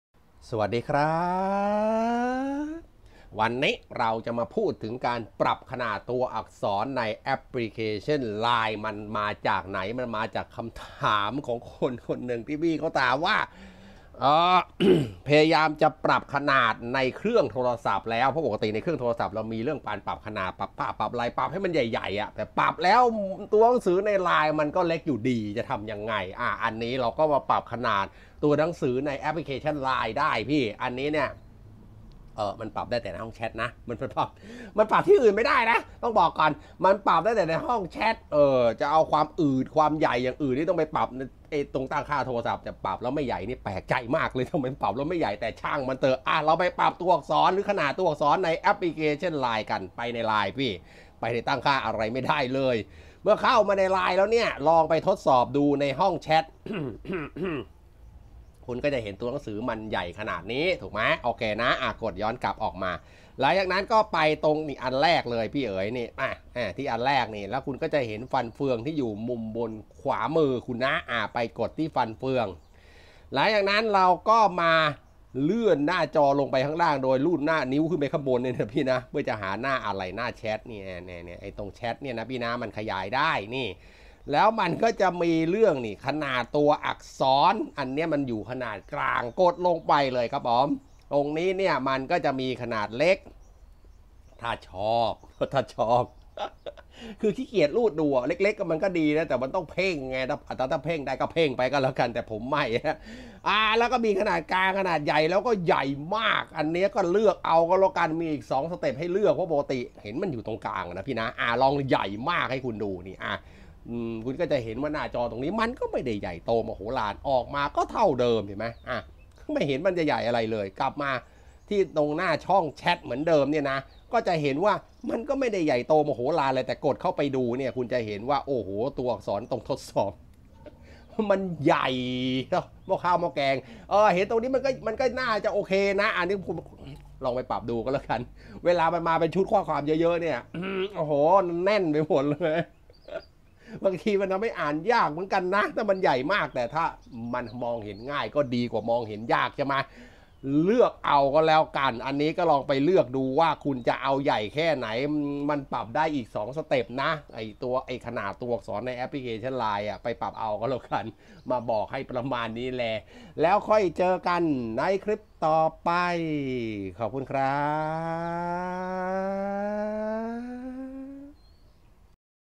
ปรับขนาดตัวอักษรในไลน์ทำไง
อันนี้เริ่มต้นจากคำถามที่มีคนถามมาว่า ปรับขนาดตัวอักษรในโทรศัพท์แล้ว แต่ว่าตัวอักษรในแอปพลิเคชัน LINE ไม่เห็นใหญ่ขึ้นเลย จะทำยังไงดี
จะทำให้ตัวอักษรใน app LINE ใหญ่ขึ้นเห็นชัดๆทำยังไง